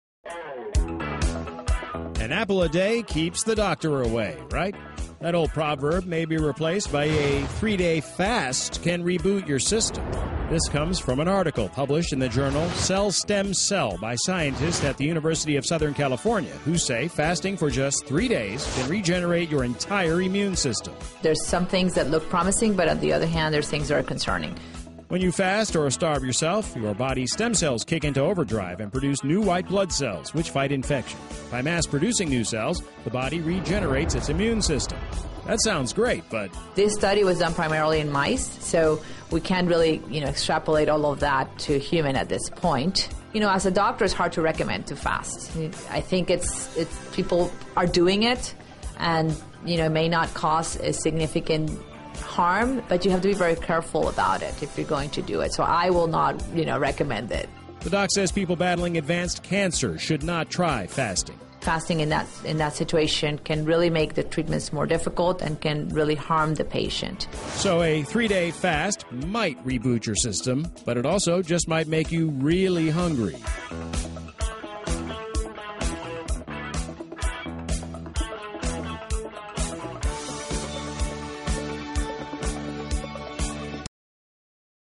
3 - day fast might reboot your immune system
An article published in the journal Cell Stem Cell by scientists at the University of Southern California who say fasting for just 3 days can regenerate your entire immune system.